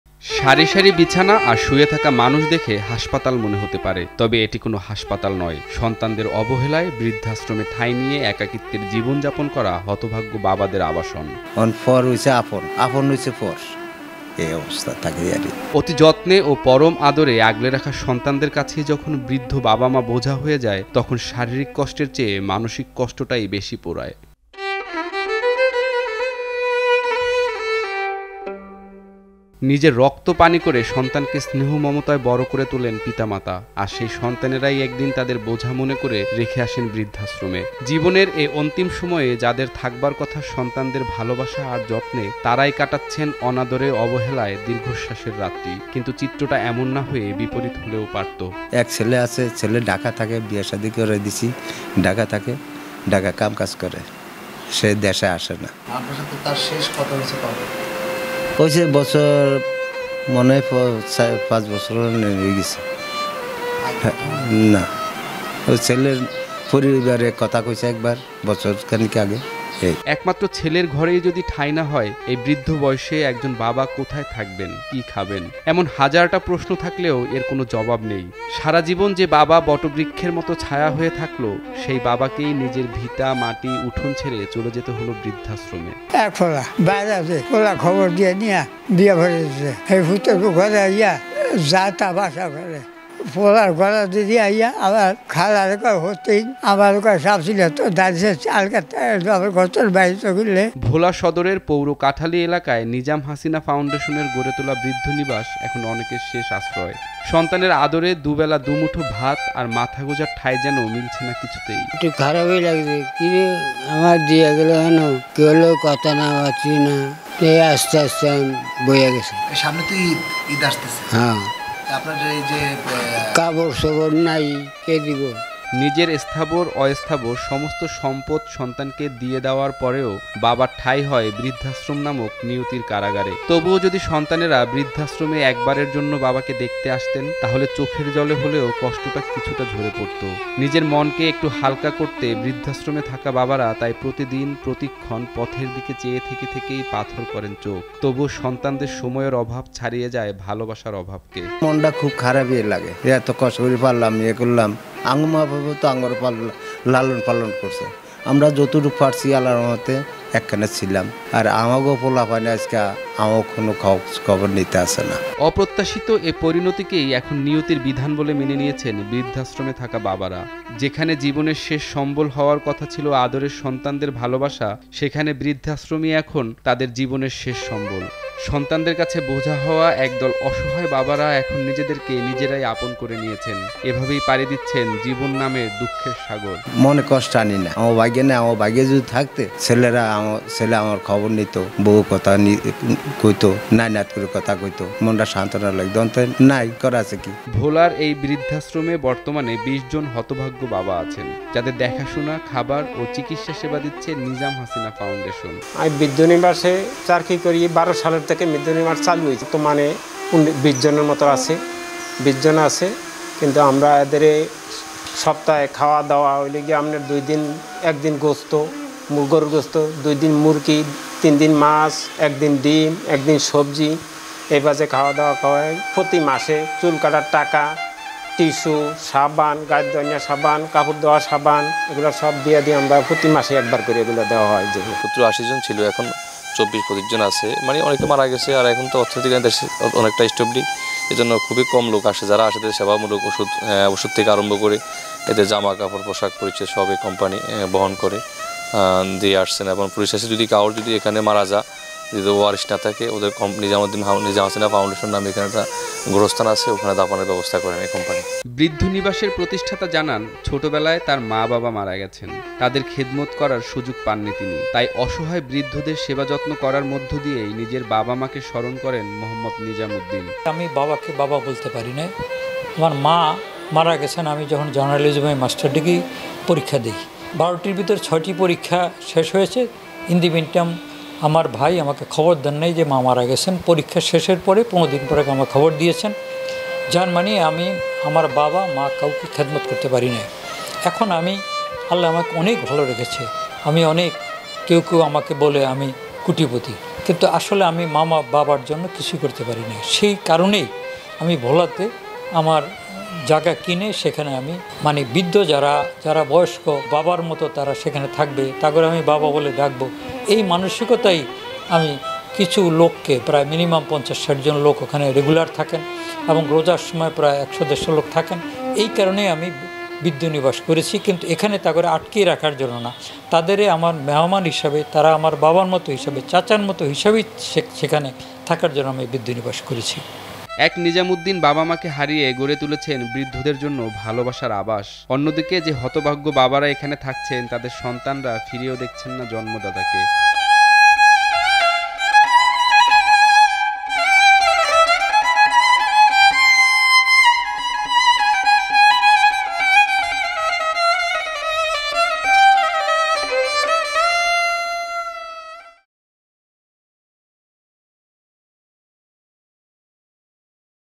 আপনজন থাকতেও একাকিত্বে বৃদ্ধাশ্রমে কাটবে তাদের ঈদ! | Nagorik TV Special
NAGORIK TV
Address: Kha-53, Nikunja-2, Dhaka-1229